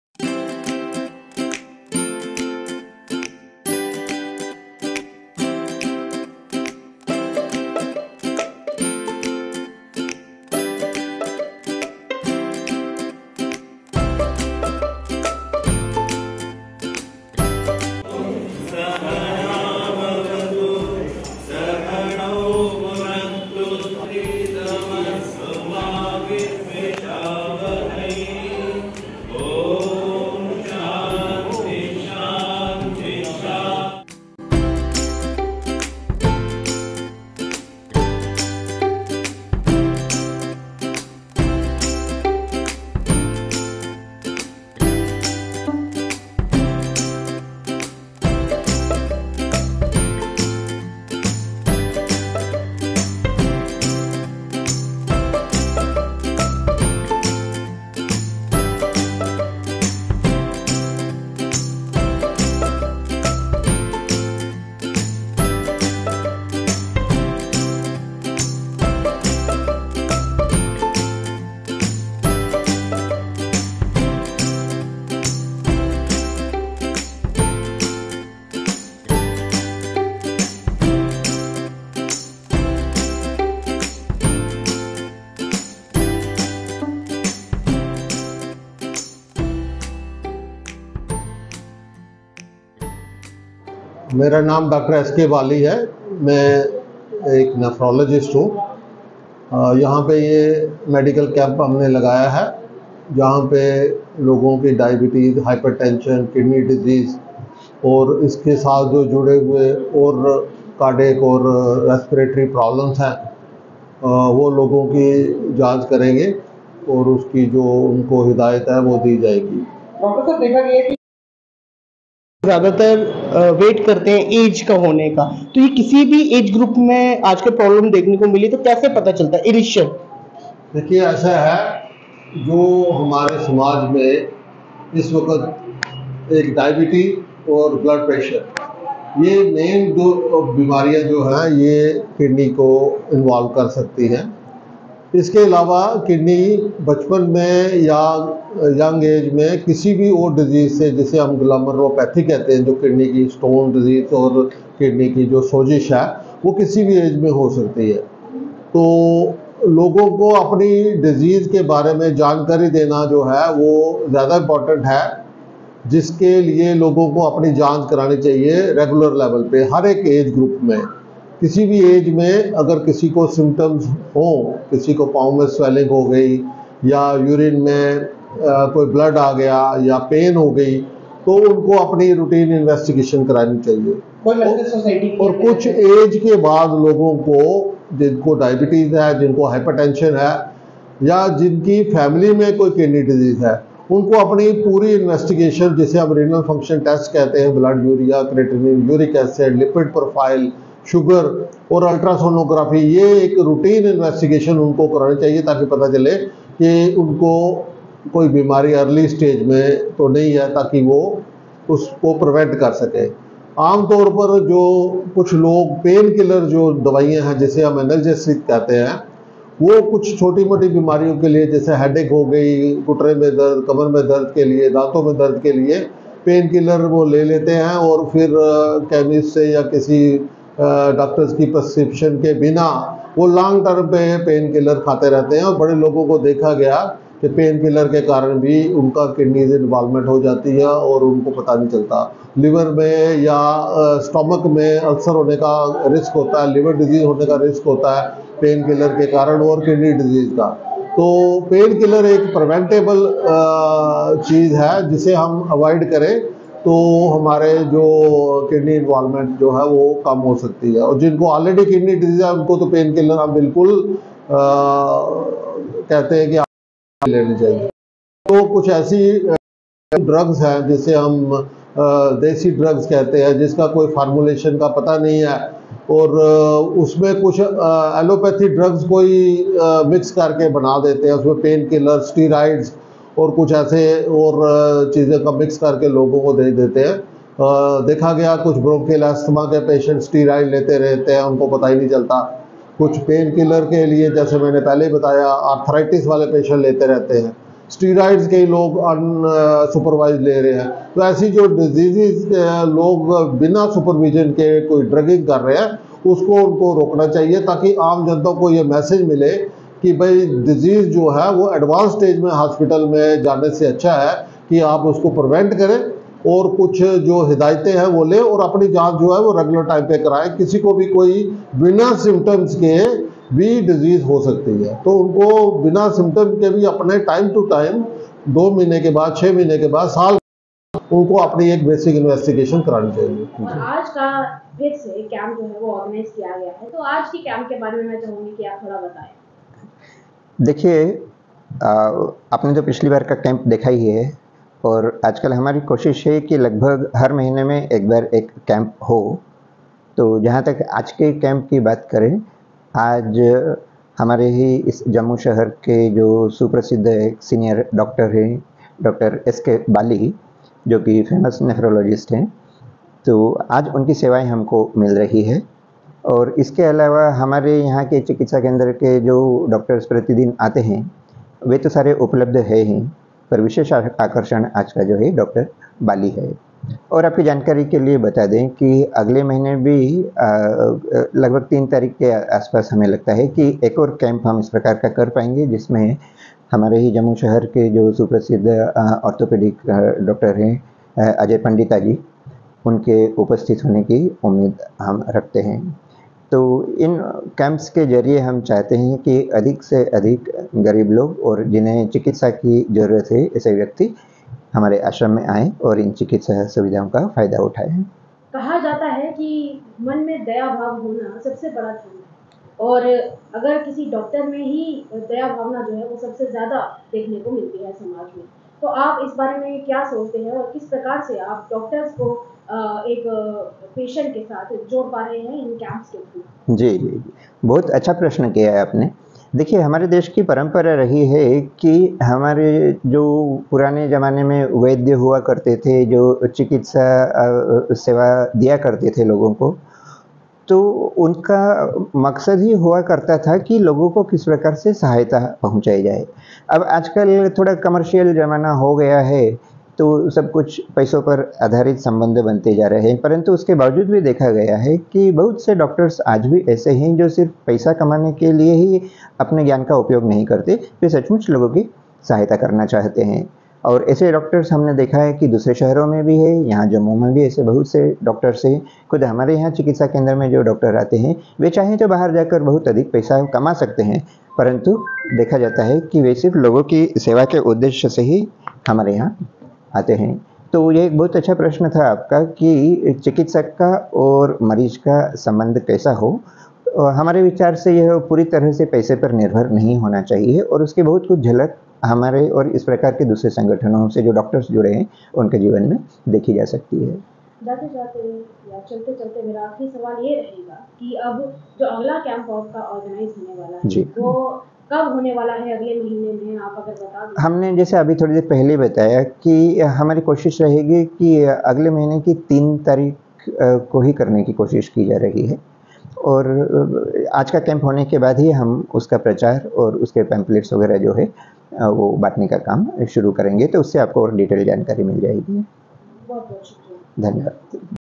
Ramakrishna Health Mission Medical Centre organises a FREE medical Camp; where Dr. S.K. Bali present
Ramakrishna Health Mission Medical Centre organises a FREE medical Camp; where Dr. S. K. Bali(kidney specialist) gave a special message, kindly watch